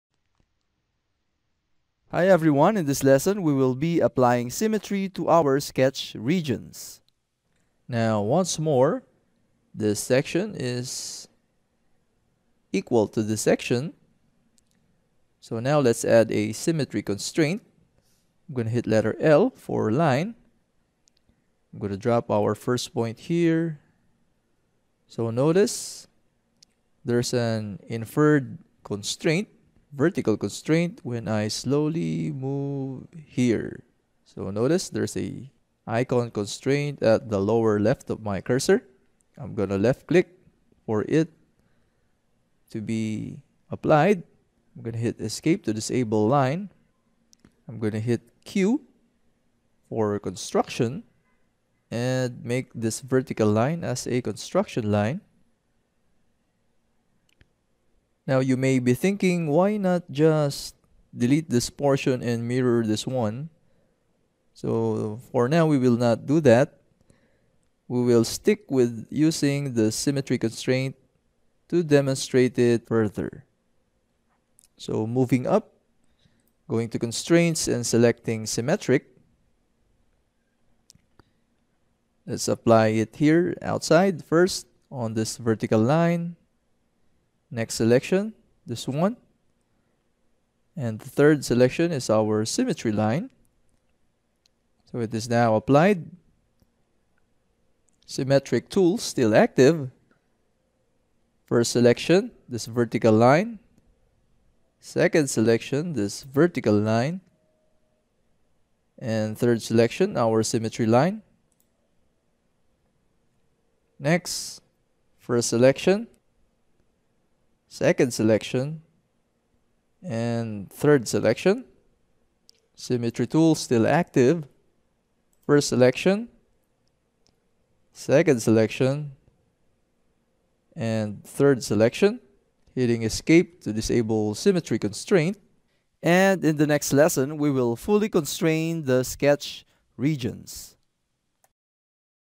Applying Symmetry to Sketch Regions
In this excerpt from JMRC'S Udemy course "Parametric Part Modeling in Onshape" course, we will learn how to apply a symmetry constraint to a sketch region.